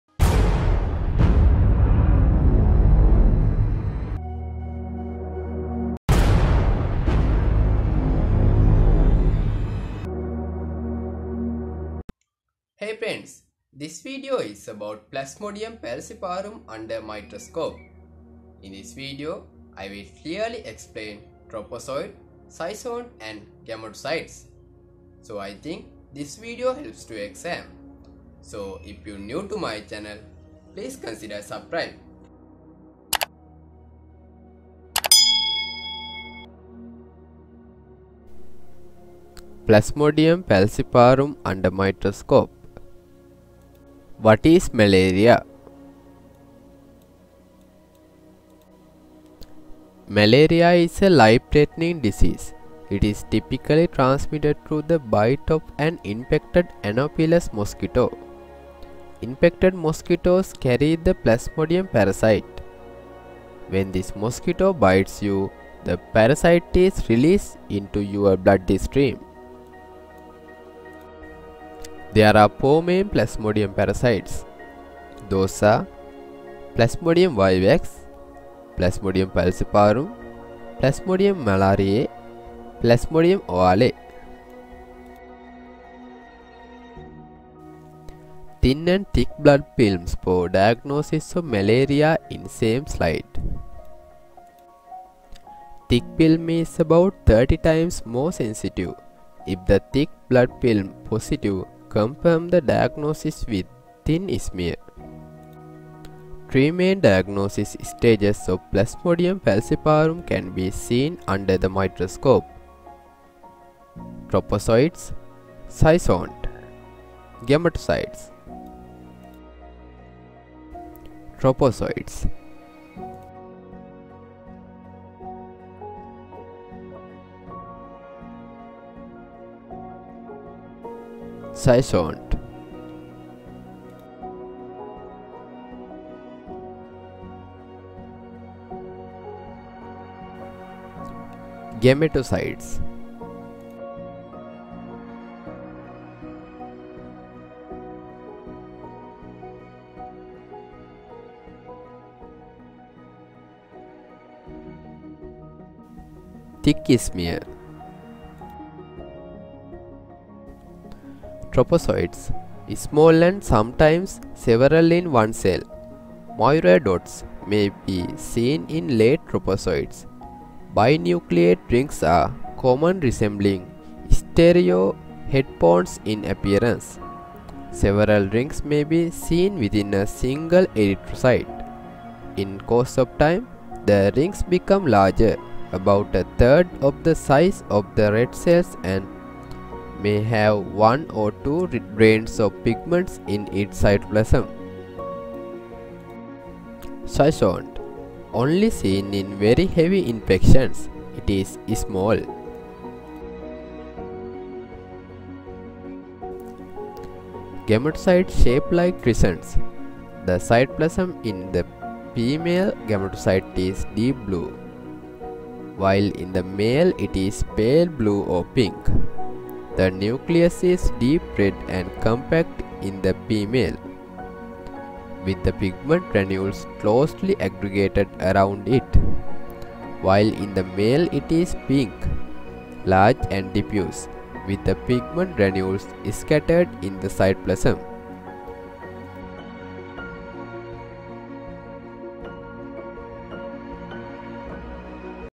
plasmodium falciparum under microscope - Trophozoites,Schizont,Gametocytes(clear explain)
Hey friends

I'm medical laboratory scientist.This video has information about plasmodium falciparum under microscope - Trophozoites,Schizont,Gametocytes(clear explain)

- what is malaria
- causes mosquitoes for malaria
- causes plasmodium parasites
- plasmodium falciparum morphology
- picture of malaria parasite under microscope

If you like my video